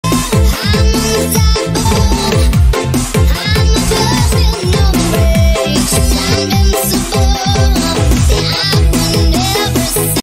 These 5 transfers could happen this winter 💥😯
Gavi -- Bayern Munich
Roberto Firmino -- Juventus
Rafael Leão -- Chelsea
Marco Asensio -- AC Milan
Toni Kroos -- Manchester City